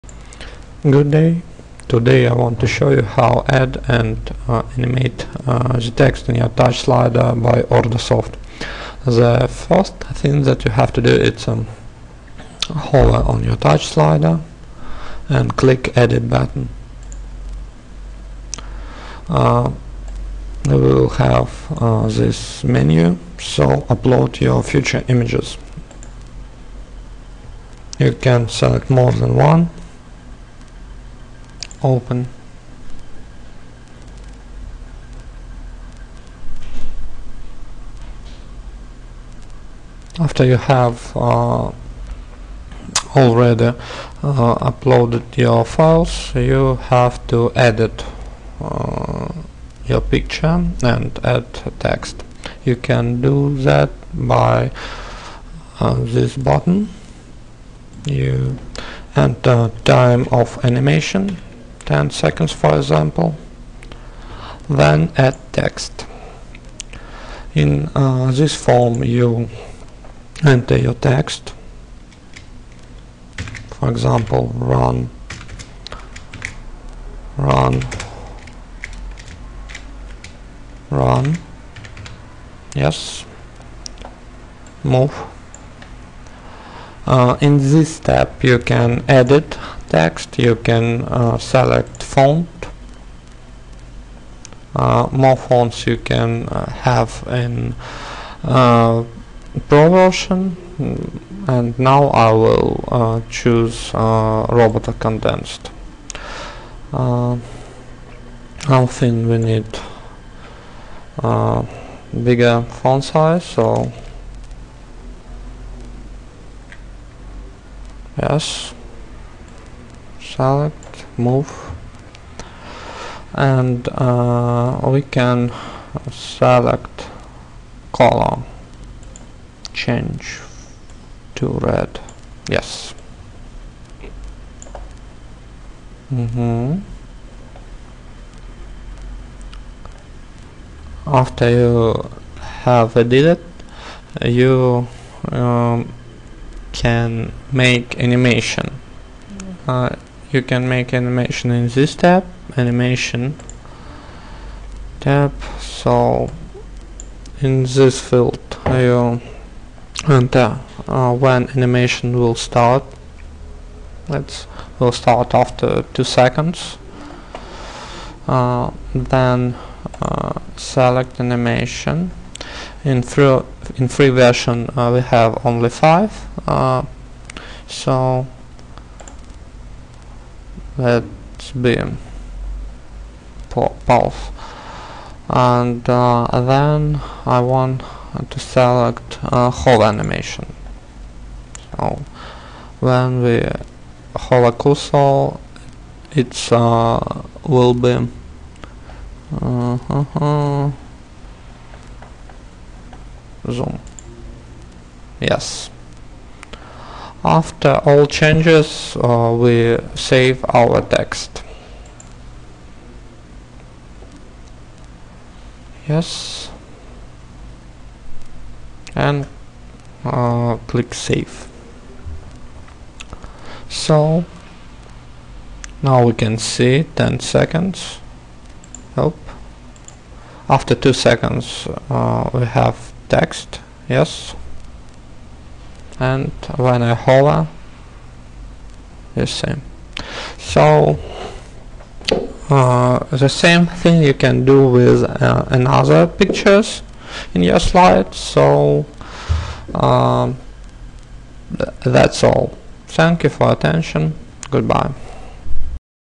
Add animate text to OrdaSoft Free Joomla Slider - free responsive joomla slideshow
This video we will demonstrate: how to add text to Free Joomla Slider extension on Joomla website.
There we also show how add animation effects to text in image slider
More information about "how create animation text slider" you can find there